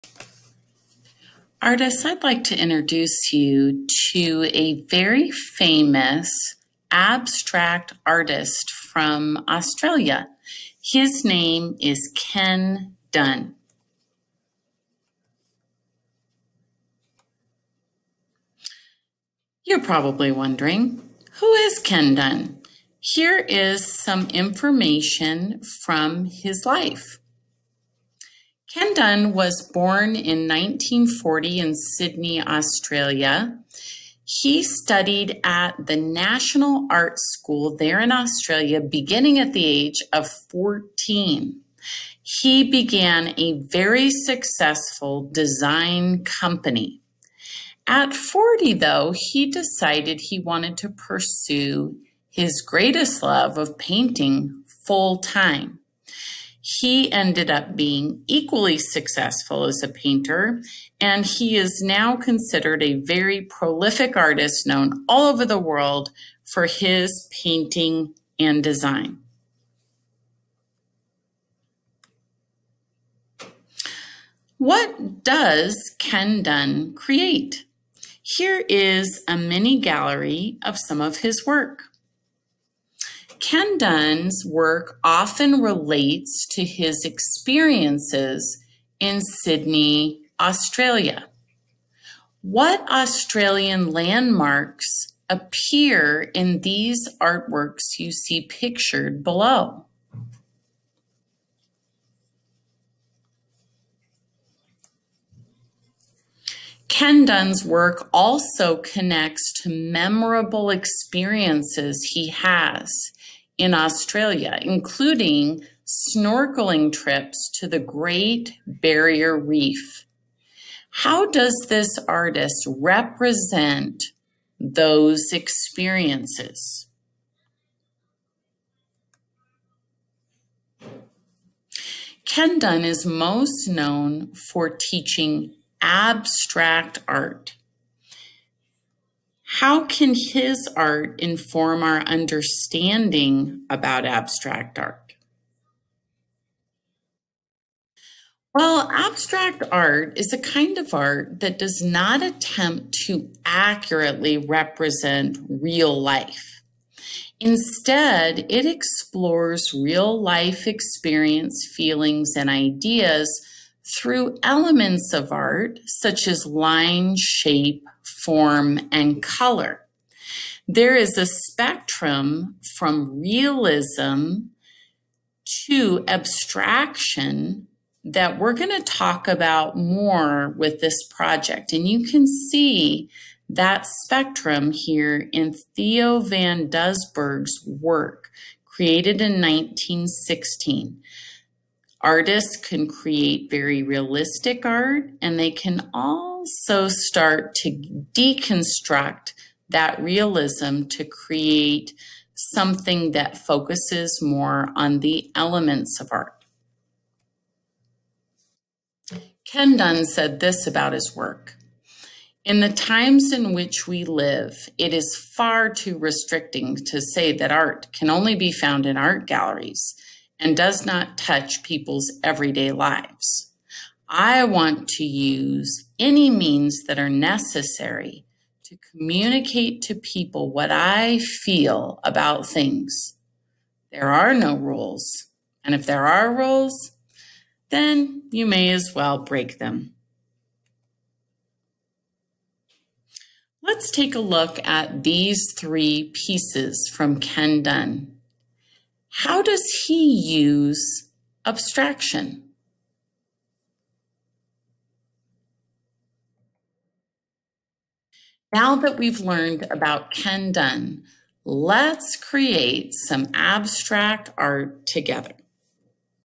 Ken Done Presentation
Students learn background information about Ken Done in preparation for an abstract art project.